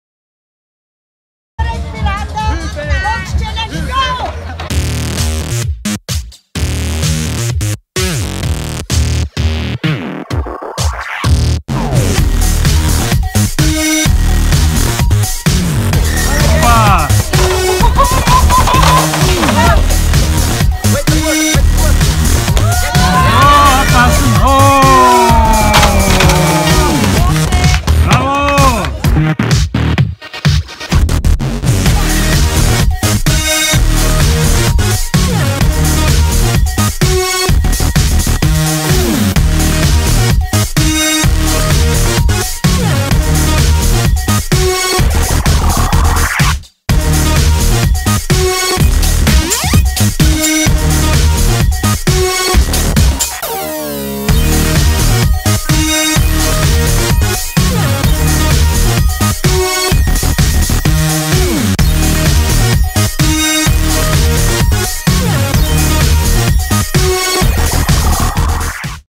Direktberater Box Challenge St Veit/Gölsen.
Direktberater Box Challenge zu Gunsten der Möwe Kinderschutzzentren. Finanziert werden dadurch Präventions Workshops an den Schulen zum Thema "Sexuelle Misshandlung, Gewalt in den Familien"